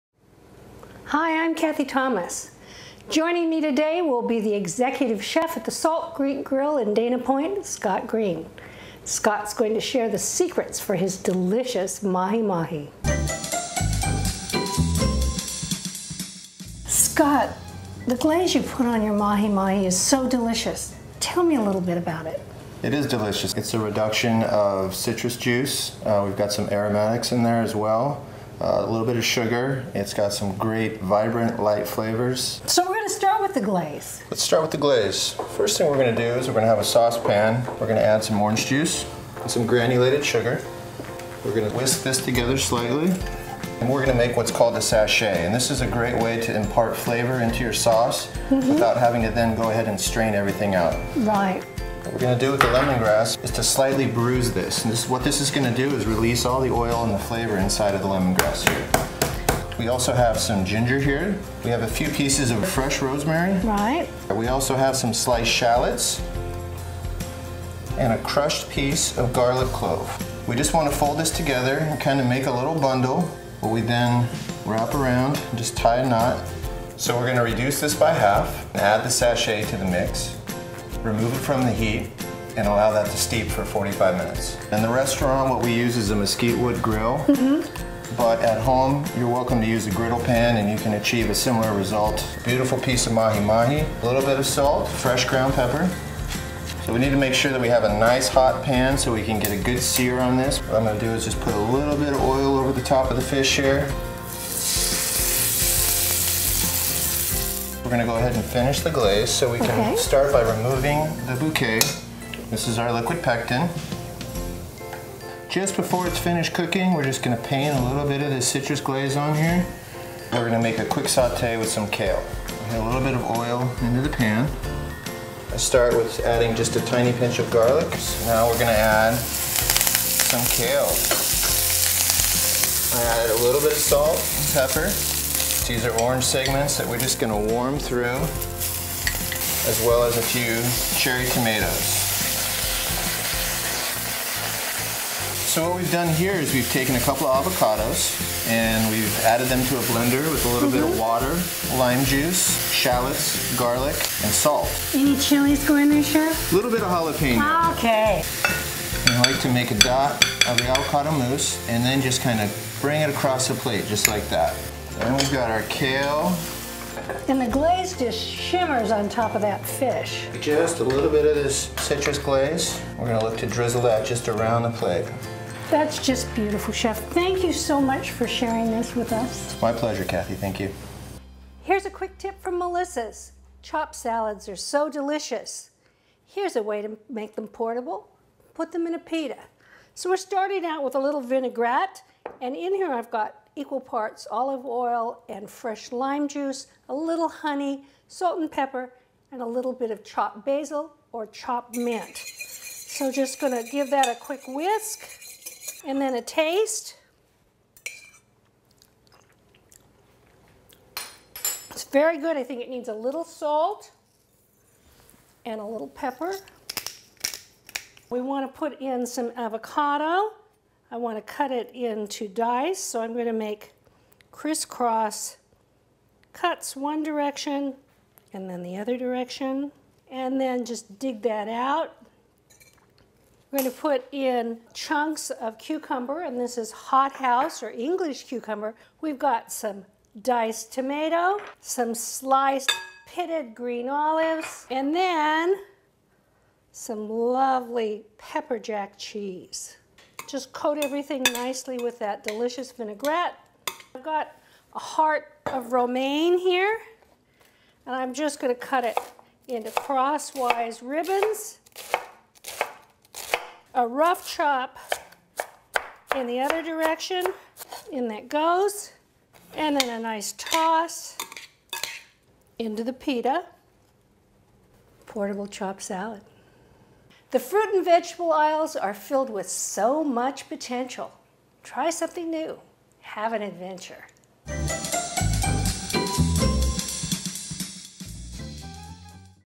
Scott Green Makes Citrus Glazed Mahi Mahi
Scott Green, Executive Chef of Salt Creek Grille, joins Cathy Thomas to make citrus glazed mahi mahi.